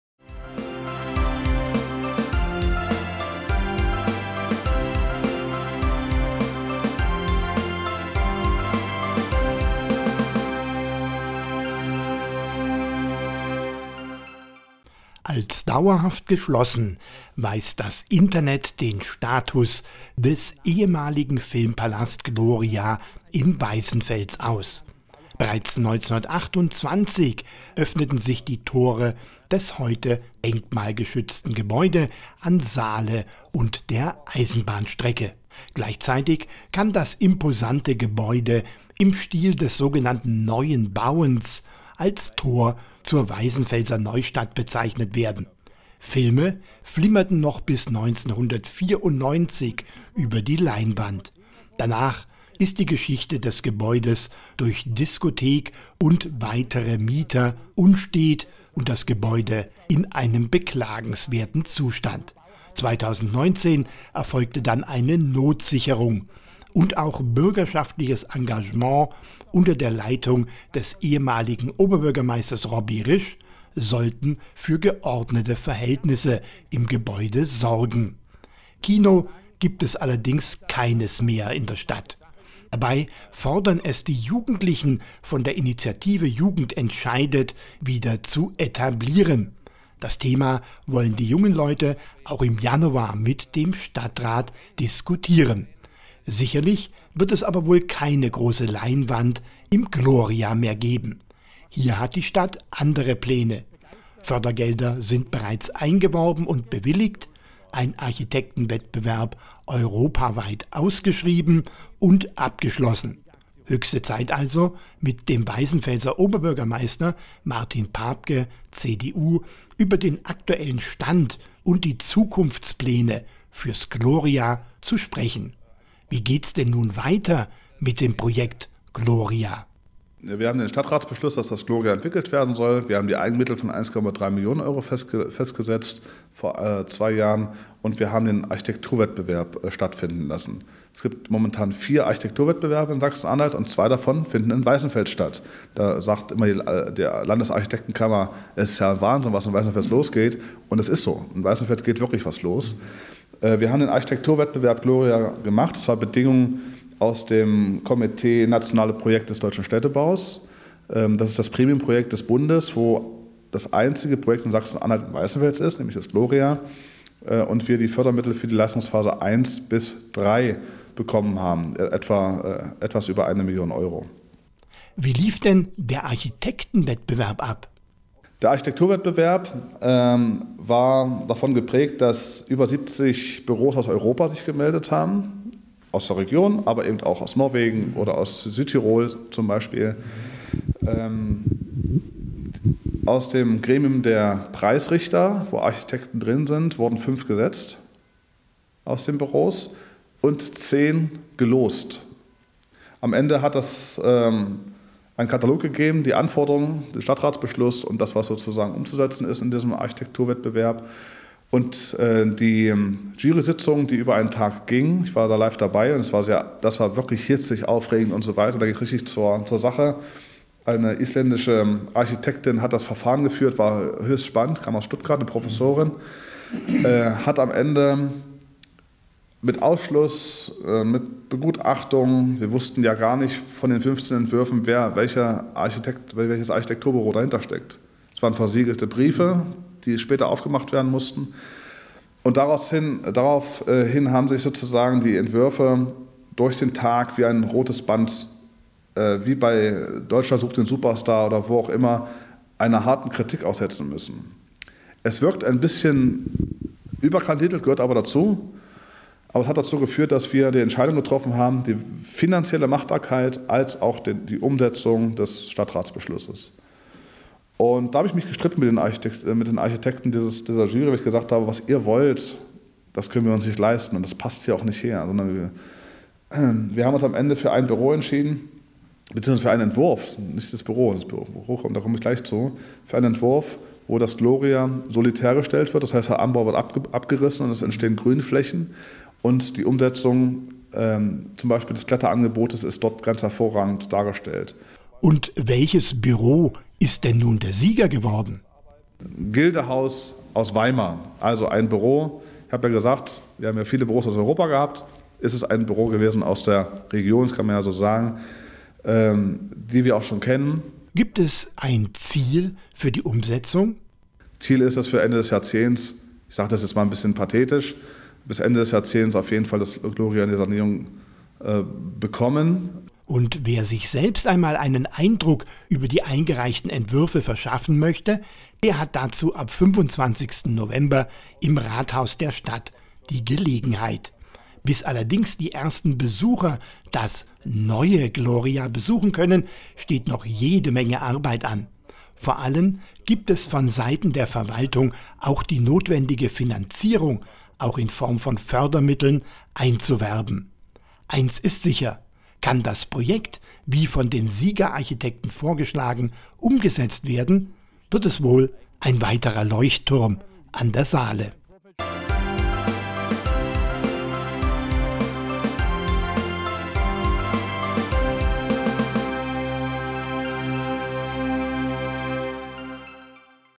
Was wird aus dem Gloria in Weißenfels?
Schwimmhalle und Einkaufsmarkt in der Altstadt sind nur zwei wichtige Themen, die derzeit in der Stadt Weißenfels nicht nur von der Politik diskutiert werden. Auch die Frage, was wird aus dem ehemaligen Kino Gloria am Tor zur Neustadt. Mittlerweile liegt das Ergebnis des Architektenwettbewerbs vor. Darüber haben uns mit OB Martin Papke unterhalten.